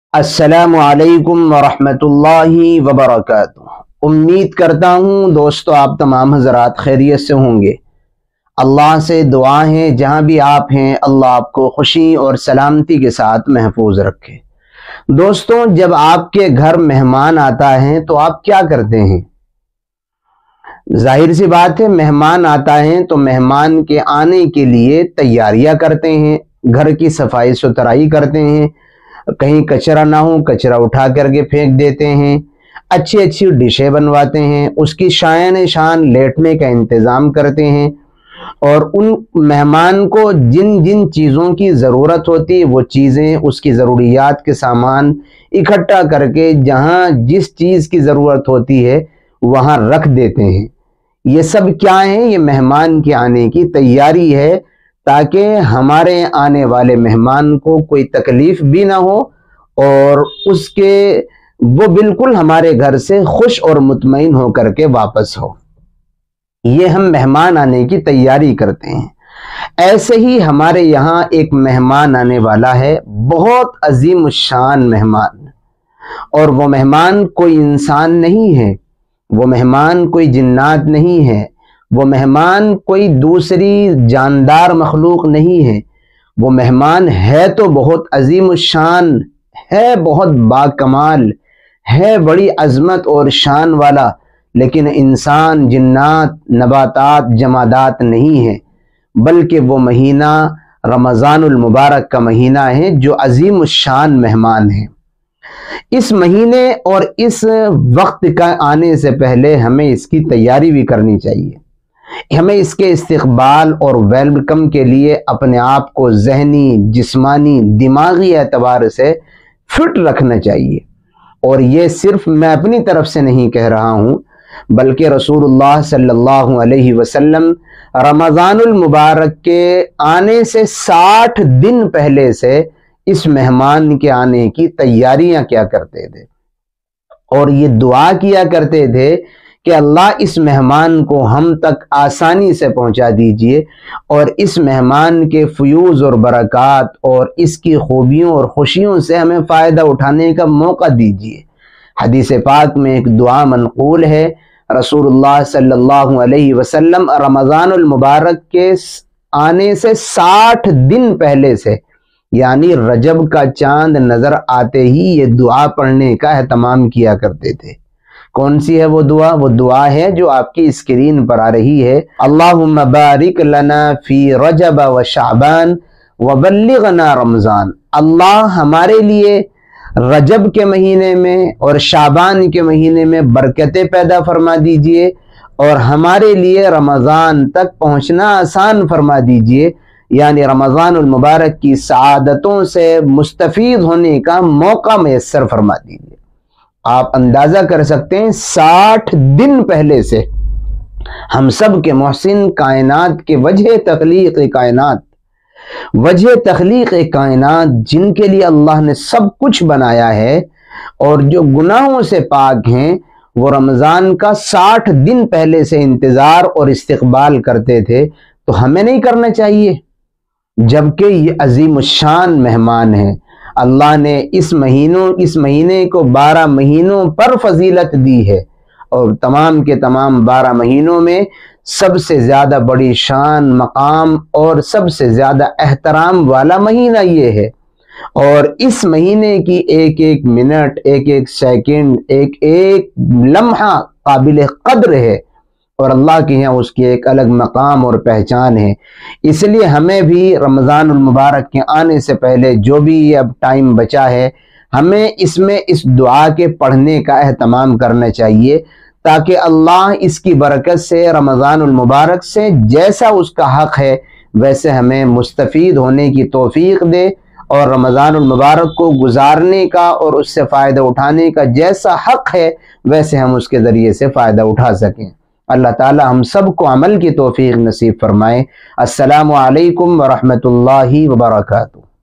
Ramzan Main Sirf 56 din Reh Gaye Ye Dua Padhna Shuru Karde ll रजब की दुआ ll Ishaat e Islam
Dosto is vedio mein hamne Rajab Ke Mahine ki Dua Bayan Ki hai Vedio Doston Se Share Kare!